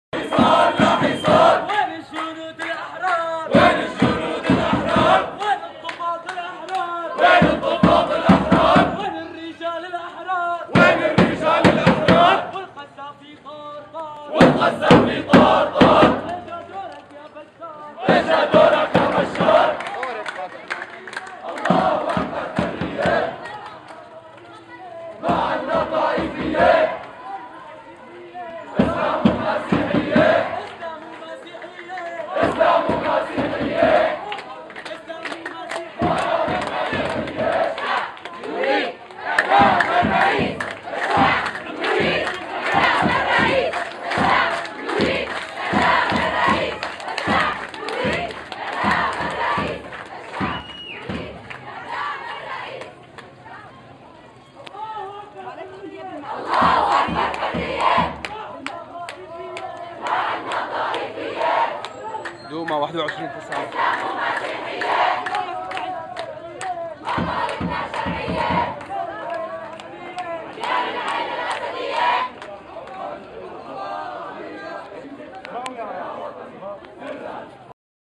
Physicians & students refuse sectarianism & regime's schools, Duma - Syria, 21 Sept 2011
please always remember to  click "cc" & the annotation buttons on the bottom left corner to see caption: English subtitles
This is from Douma city (Duma on google maps) in Damascus rural (Rif-Dimashq).
The demonstration is by physicians & university & school students.
Please notice that female protesters are usually in the middle of the group to protect them from being beaten, insulted, shot, or abducted by security & Shabiha (regime-sponsored gangs)